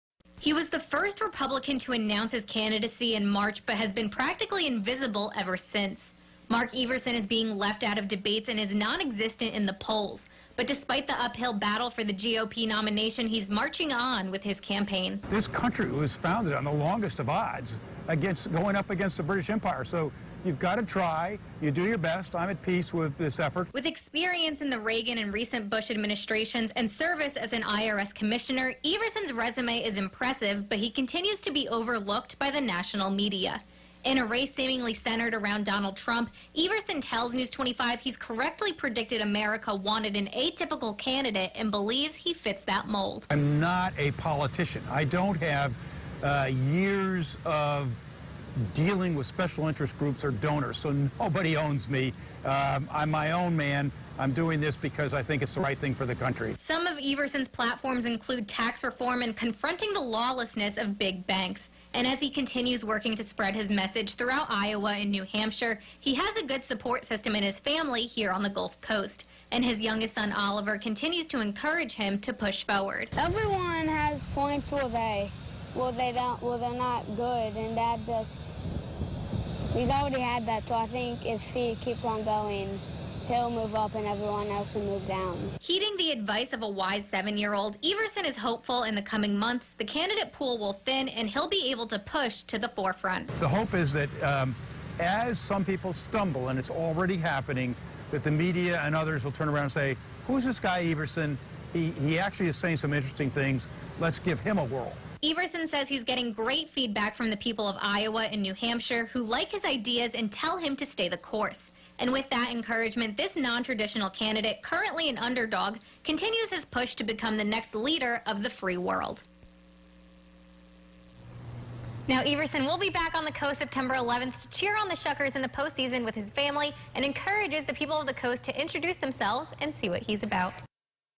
WXXV: President Hopeful Mark Everson on the Coast
The Mississippi Gulf Coast's WXXV caught up with Mark during a stop back home before his next New Hampshire Swing